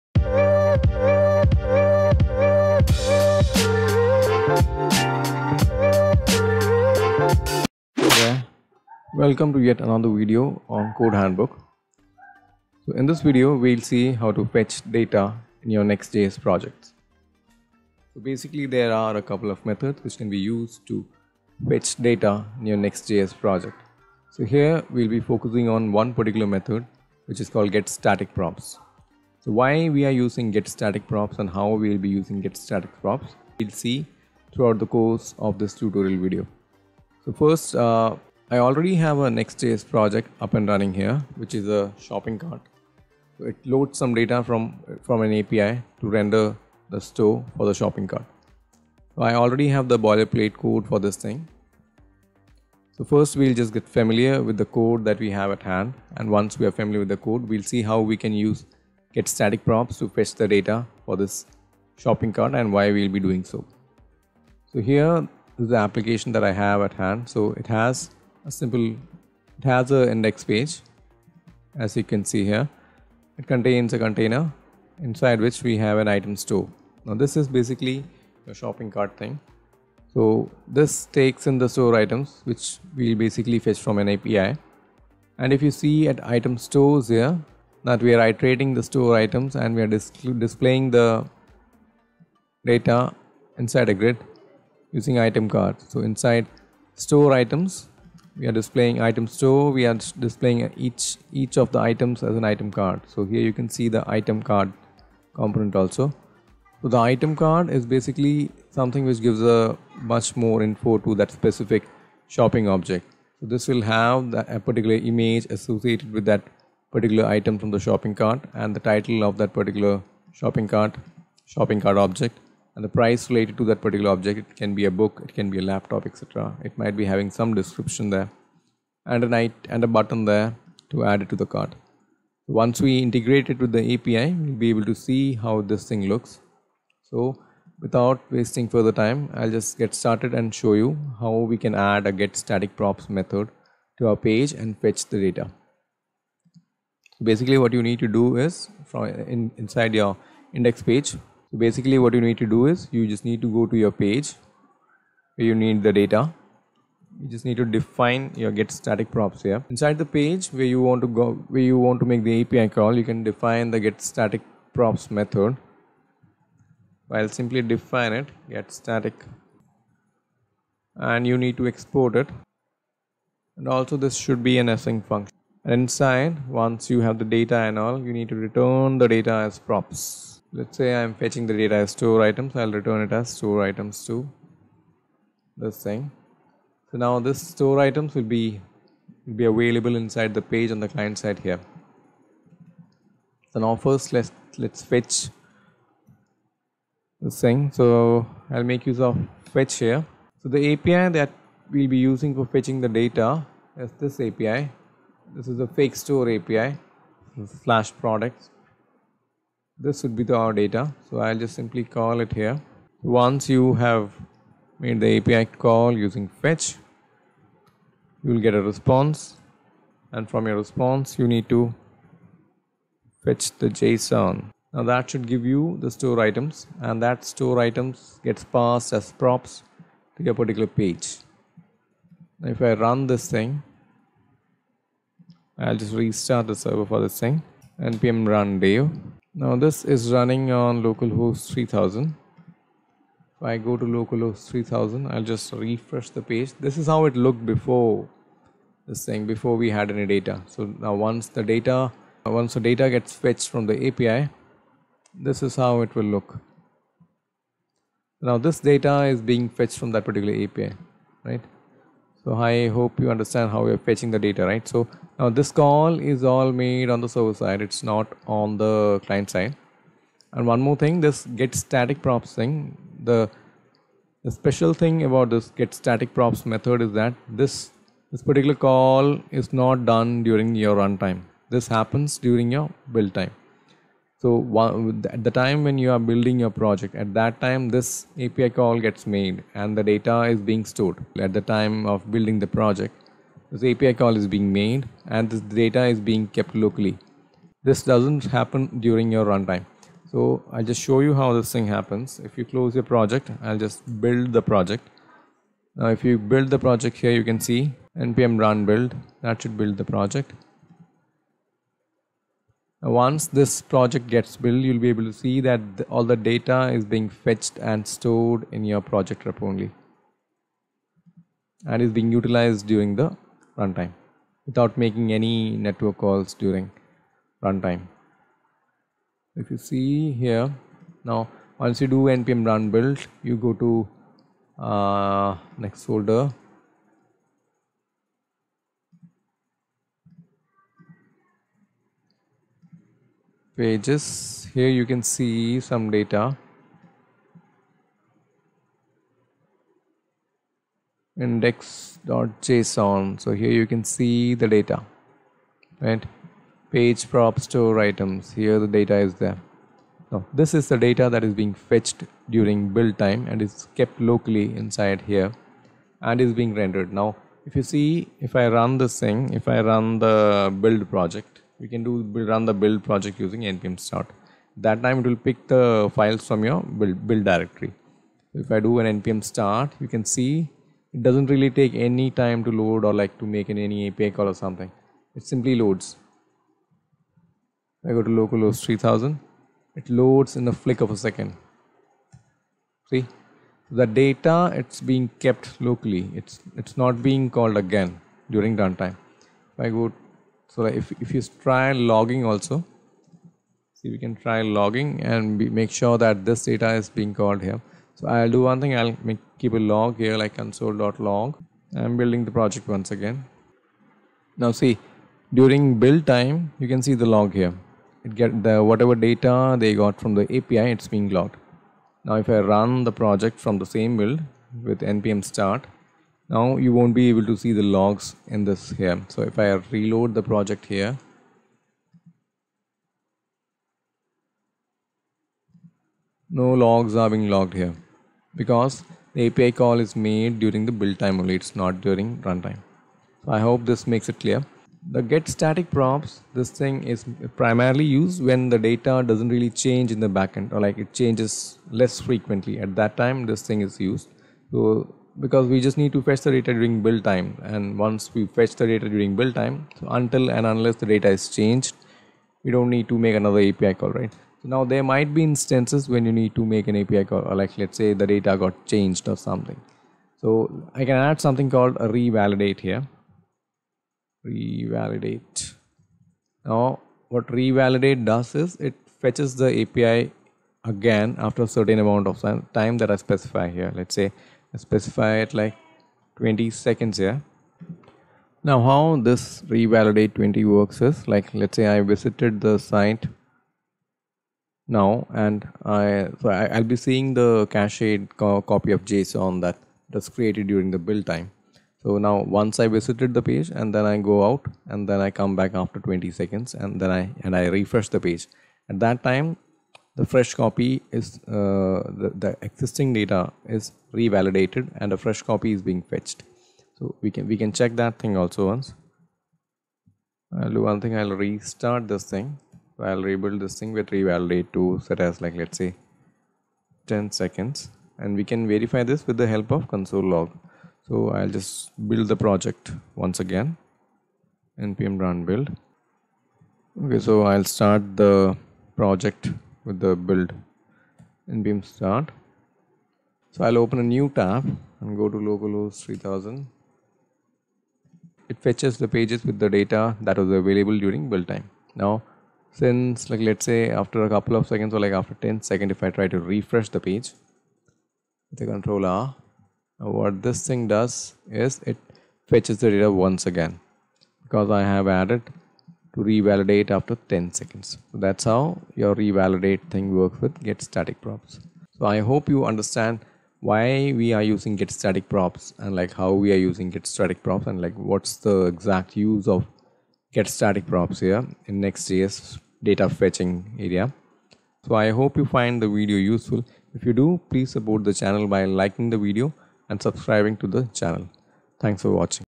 Data Fetching in Next.js Using getStaticProps | Fetch Data From REST API | Next.js Tutorial
In this Next.js project tutorial, you'll learn how to fetch data in Next.js using getStaticProps. The video focus on why you need getStaticProps and how to use getStaticProps for fetching data from the REST API using fetch.